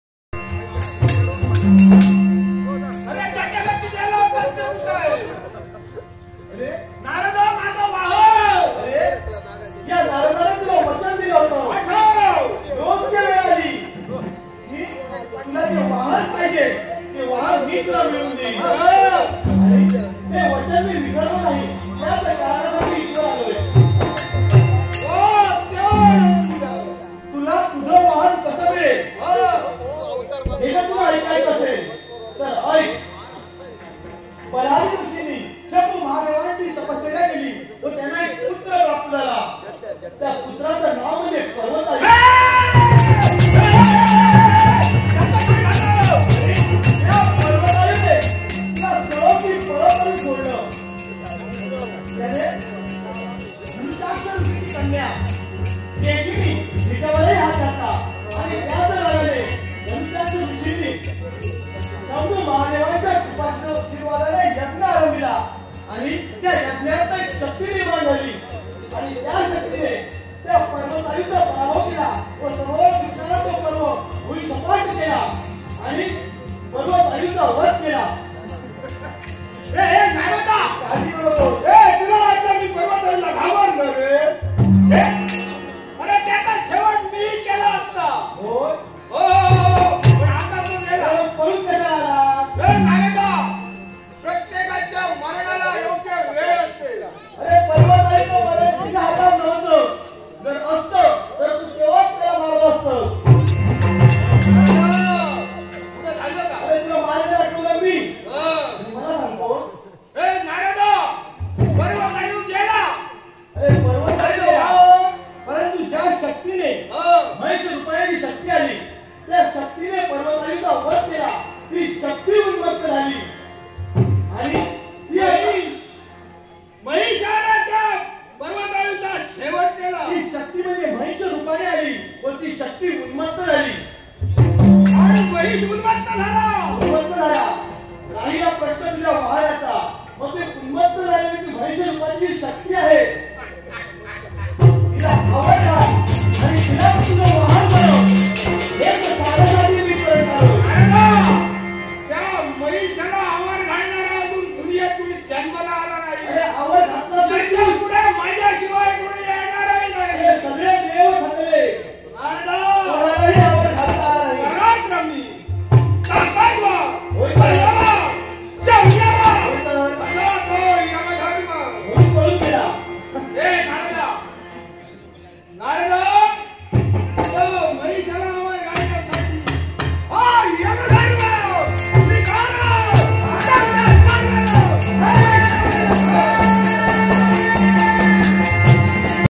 ट्रिकसीनयुक्त दशावतारी नाटक-'धर्म म्हैस्य वाहनम्' (श्री देवी भावई दशावतार नाट्य मंडळ कुणकेरी)
# श्री देवी भावई दशावतार नाट्य मंडळ कुणकेरी
# महान पौराणिक ट्रिकसीनयुक्त दशावतार नाटक-'धर्म म्हैस्य वाहनम्'
@# पात्र परिचय -
यम-श्री दीपक झोरे
नारद-श्री महादेव गावडे
शंकर-श्री एकनाथ सावंत
बलाधीऋषी-भूषण सावंत
धनुचाक्श ऋषी- कृष्णा गावडे
मेधावती-श्री मदन भैरवकर
केशनी-श्री तेजस देसाई
पर्वतायु-श्री बुधाजी गावडे
ब्राम्हराक्षस-श्री जितेश कुणकेरकर
भट-श्री भरत सावंत
भटीण-श्री भिवा सावंत
महिश-श्री प्रकाश गुरव
# संगीतसाथ -
हार्मोनियम-श्री प्रसाद सावंत,श्री नाना सावंत
पखवाज-श्री मंदार मेस्त्री
झान्ज-श्री महेश मेस्त्री